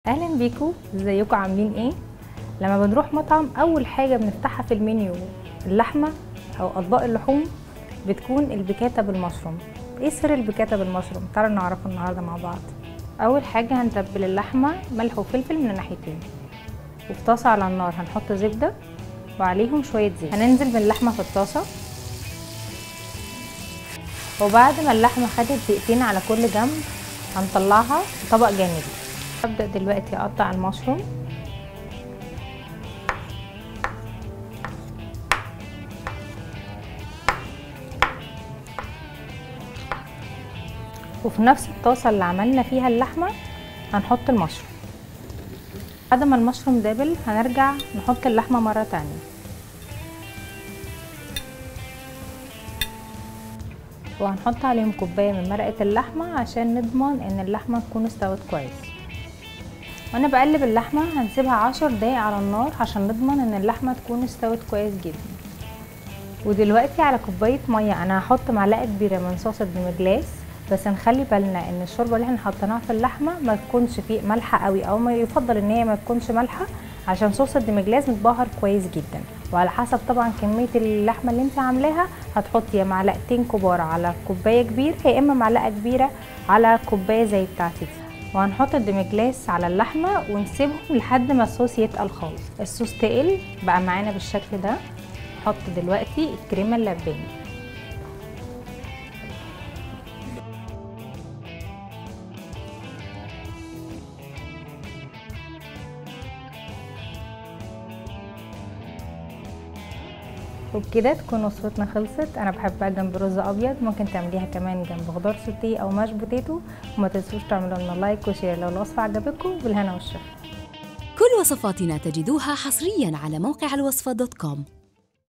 طريقة عمل اسهل واطعم بيكاتا لحم بالصوص البني - ELWASFA
المقادير
1/2 كيلو لحم بيكاتا
2 كوب مشروم طازج
رشة ملح
رشة فلفل أسود
2 معلقة كبيرة صوص ديمي جلاس
1/4 كوب كريمة لباني
2 كوب شوربة لحم
2 مكعب زبدة
2 معلقة كبيرة زيت زيتون